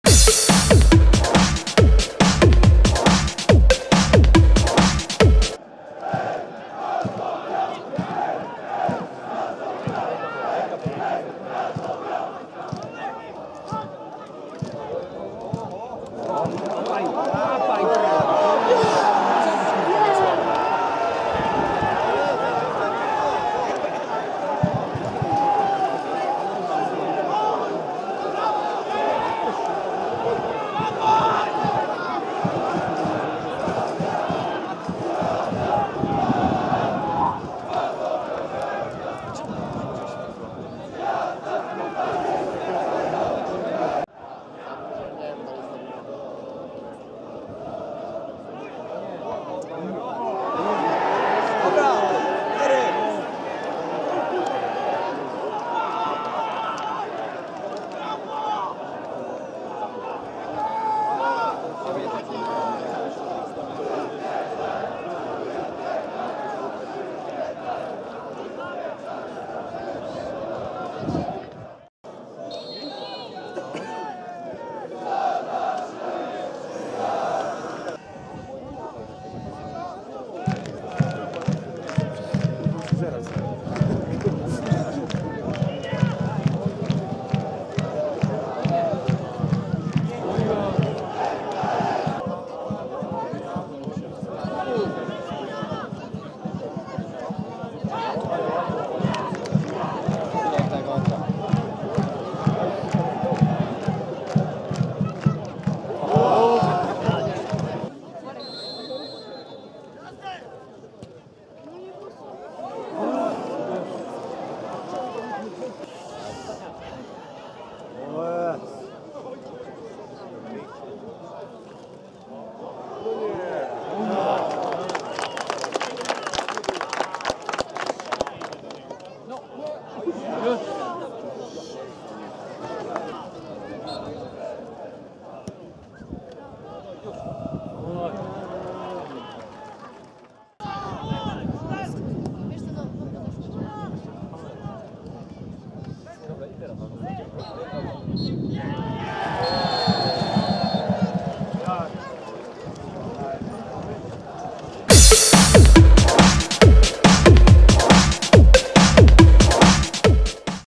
Baraż o awans do III ligi Mazovia Mińsk Mazowiecki RKP Broń Radom 0 2 Skrót
Baraż o awans do III ligi Mazovia Mińsk Mazowiecki   RKP Broń Radom 0 2 Skrót meczu

Kamil Czarnecki   9'
Kamil Czarnecki  19'

Mazovia: Łysik - Kozikowski, Grabek, Noiszewski, Janusiewicz, Herman, Romanow, Puciłowski, Bondara, Tokaj, Żurawski.

Broń: Młodziński - Korcz, Grunt, Kventsar, Wicik, Leśniewski, Sala, Imiela, Nowosielski, Putin,